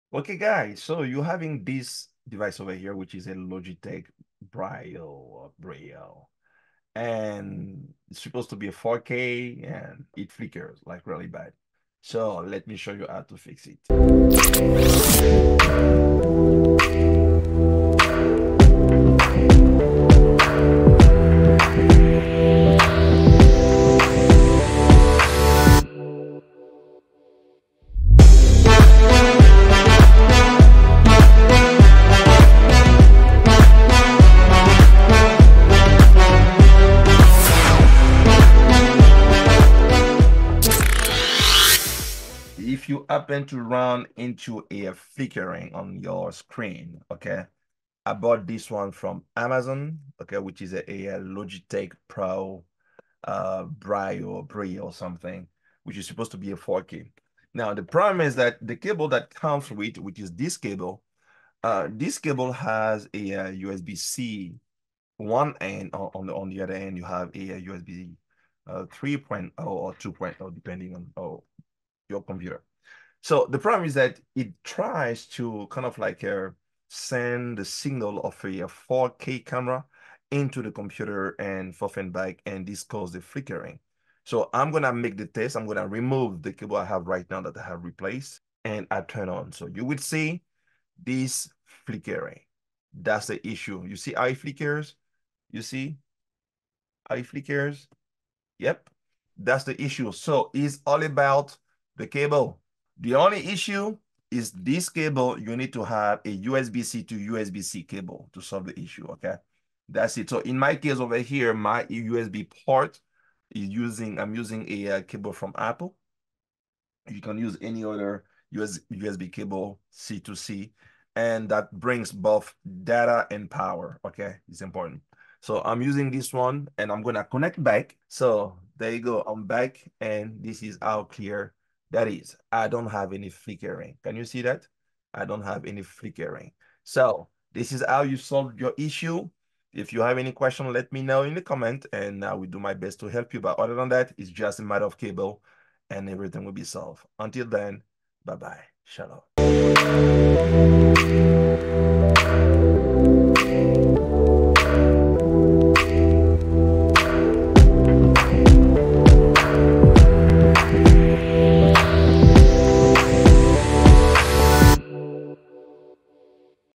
How to fix Logitech BRIO 4K with a ton of flickering – SOLVED!!!
I bought this unit from amazon and it was horribly flickering. I did not want to go through the hassle of returning, so I came up with a working solution.. in this case, it's just changing the cable using the USB-C to USB-C on my computer. If your computer does not have a USB-C port, you may want to decrease the frame per second, and set it to 30fps instead of 60fps.